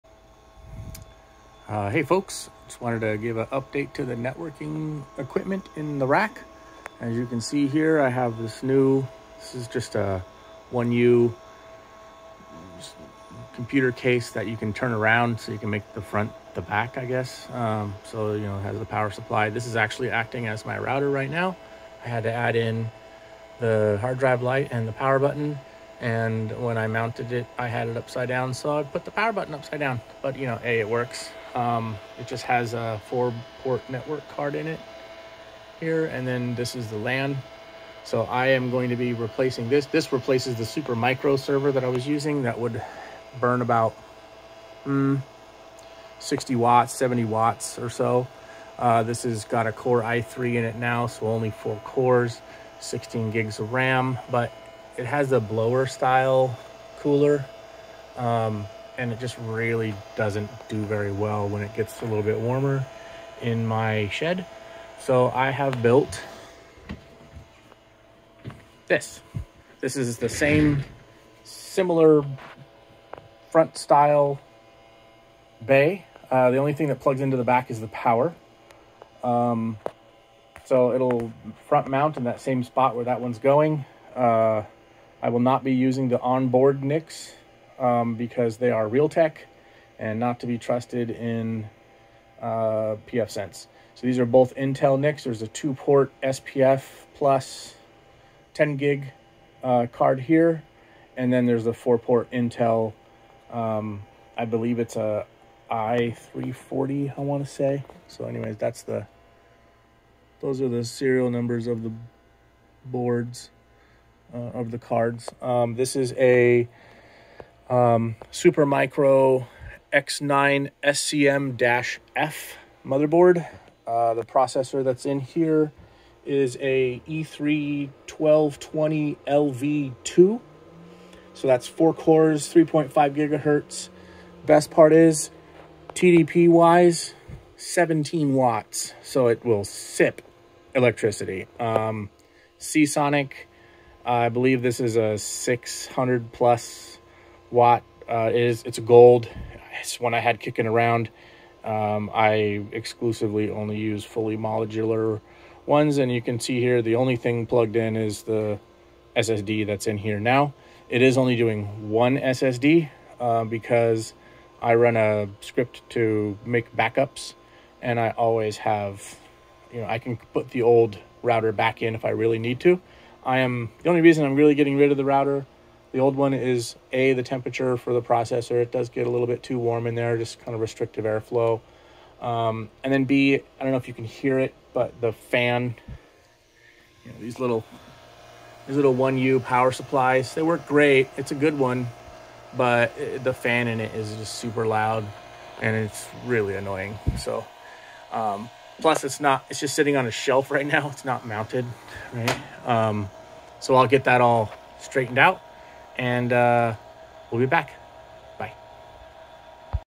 Server Rack Update "Out with the old" pfSense router replacement (Part 1 of 2)
Supermicro 1u router died and replaced with temporary Core i3 with noisy fan. Its got to go.